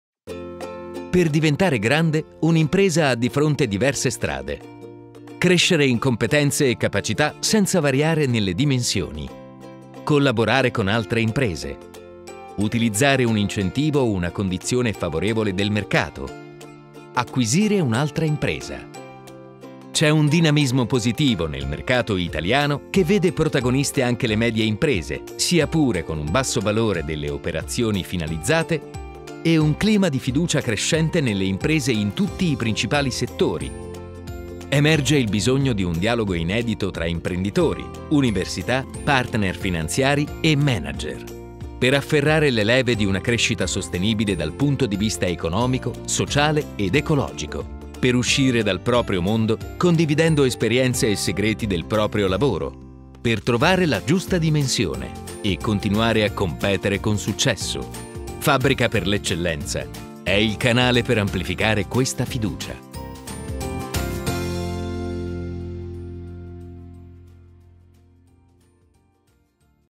Fabbrica per l'Eccellenza: la Crescita Sostenibile
L'impresa può crescere perché acquisisce nuove competenze, perché acquisisce un'altra azienda o perché sviluppa nuove opportunità in nuovi mercati collaborando con altre imprese. Al Primo Forum Annuale della Media Impresa Italiana se ne è parlato nel tavolo sulla Crescita Sostenibile: questo il video di approfondimento realizzato in collaborazione con Nomisma